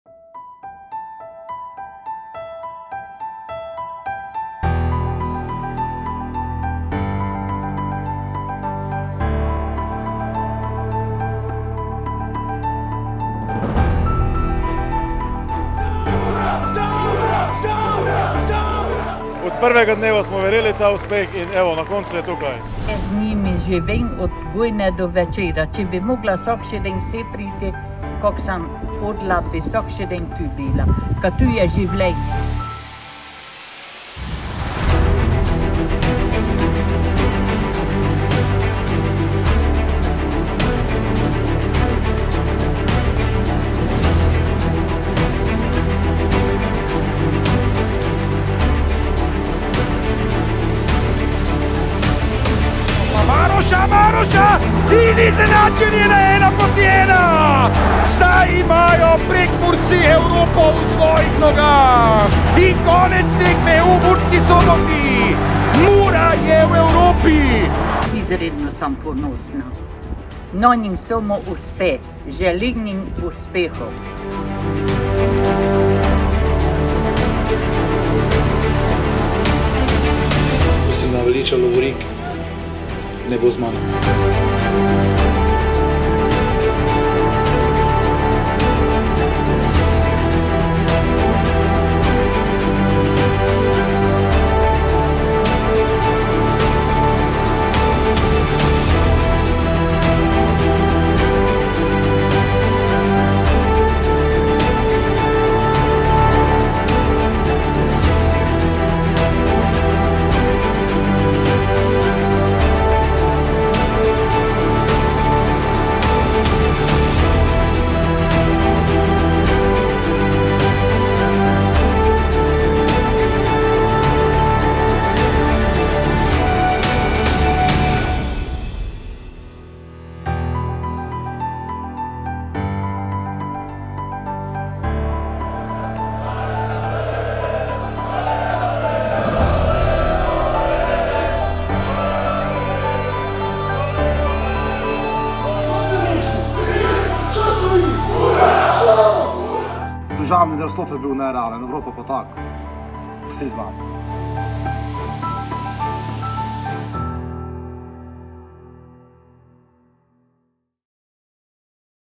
𝐎𝐃 𝟑. 𝐋𝐈𝐆𝐄 𝐃𝐎 𝐄𝐕𝐑𝐎𝐏𝐄
Z leti trdega dela so si naši nogometaši prislužili nagrado. Zahvala tudi Generali zavarovalnica da nam stojijo od začetka ob strani. Oglejte si našo pot od Odranec (2017) do Evrope (2021).